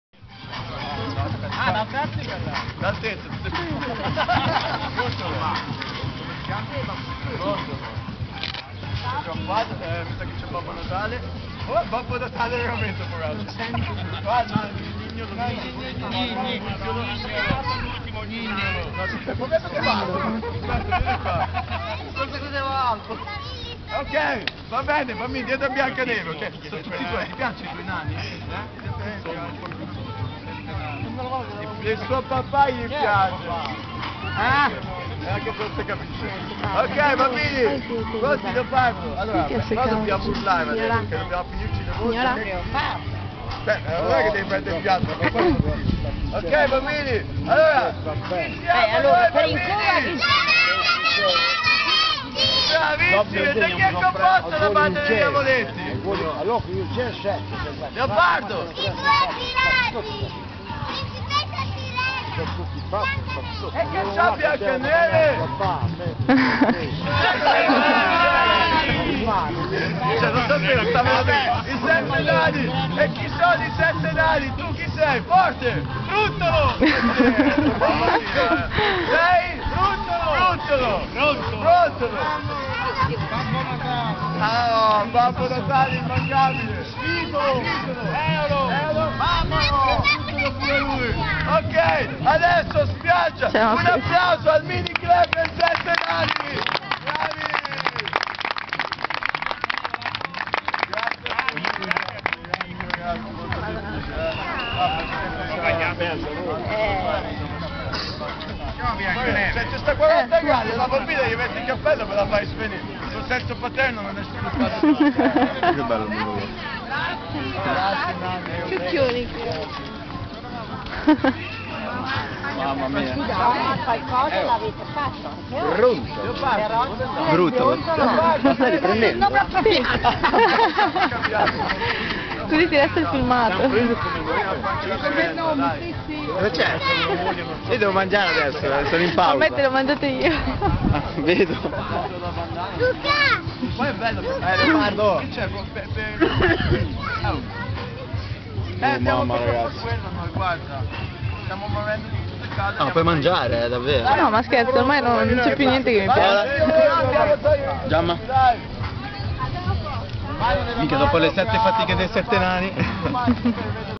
7 nani a Sharm el Sheikh
Alex e Luca si improvvisano NANI per i bambini del villaggio a Sharm el Sheikh. Divertente!!!